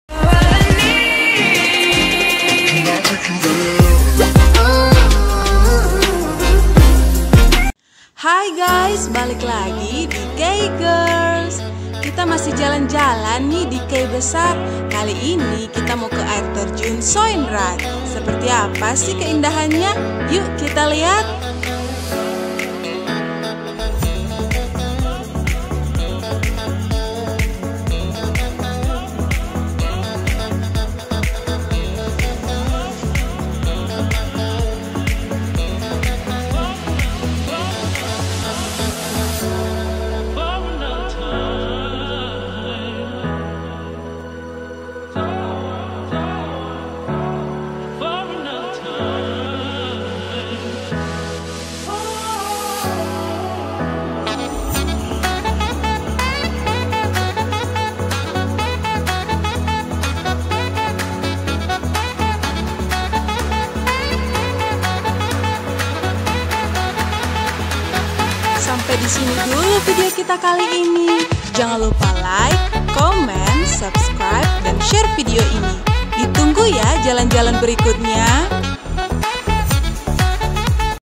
AIR TERJUN SOINRAT KEI BESAR MALUKU TENGGARA
Air terjun Soinrat yang berada di Kei Besar ini merupakan salah satu tempat wisata yang wajib kamu kunjungi di Kei Besar, tempatnya yang tak jauh dan memiliki keindahan yang begitu mempesona membuat banyak masyarakat kei besar maupun di daerah lain untuk berkunjung kesini.